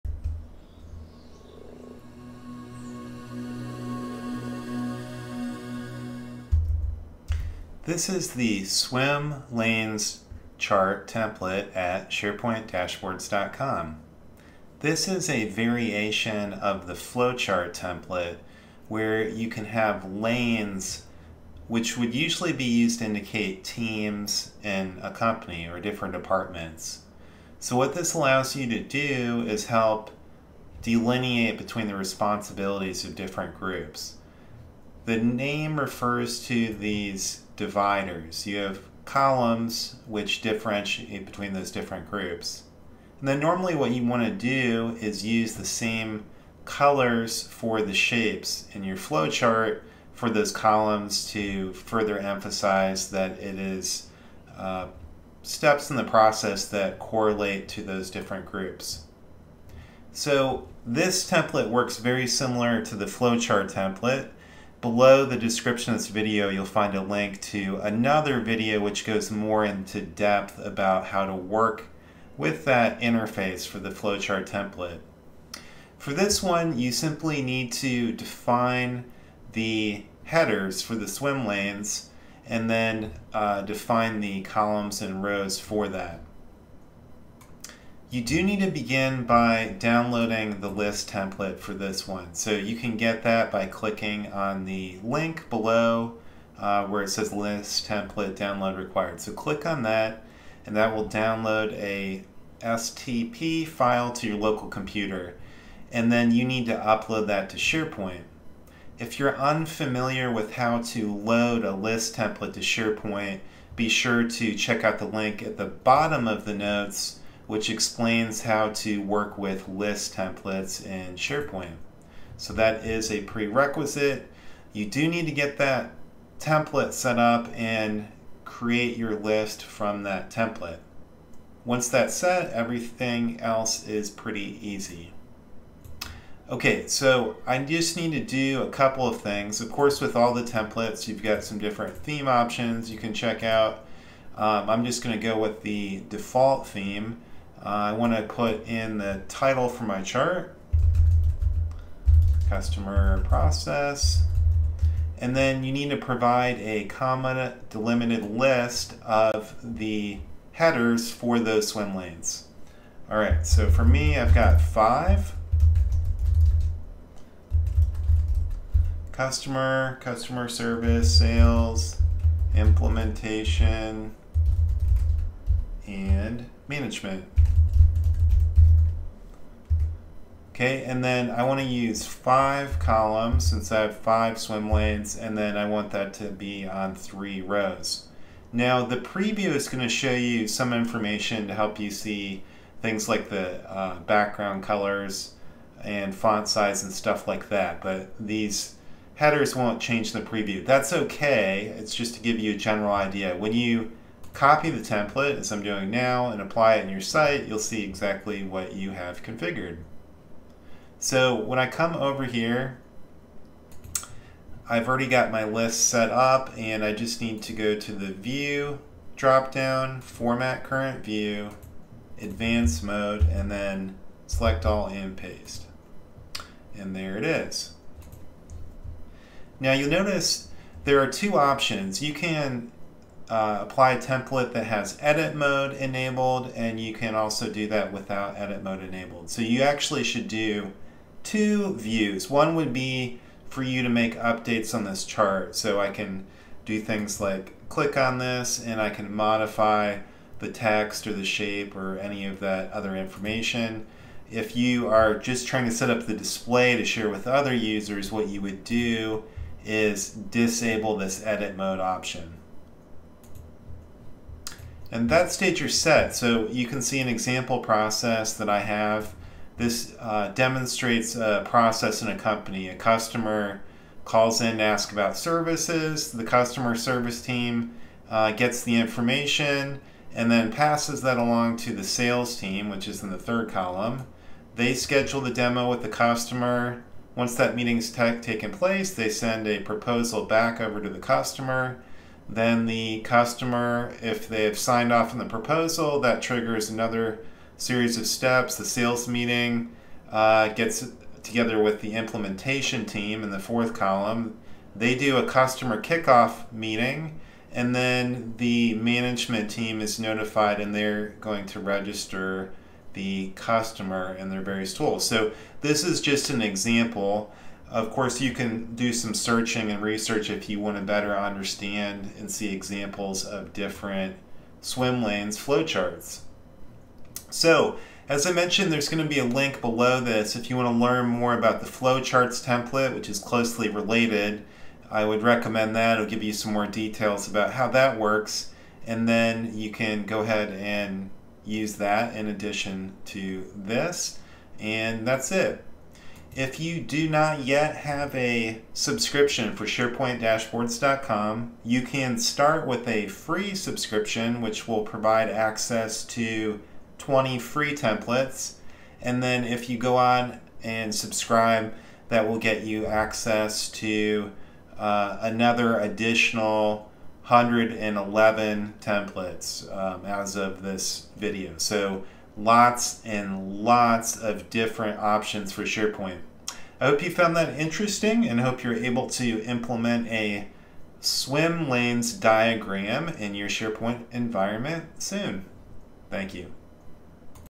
Swim Lanes Flow Chart in SharePoint Online Microsoft Lists Modern List View
Learn how to add a swim lanes flow chart to your modern SharePoint list view. This template is available here:

Be sure to check out the more detailed related video which explains more about the flow charts template here: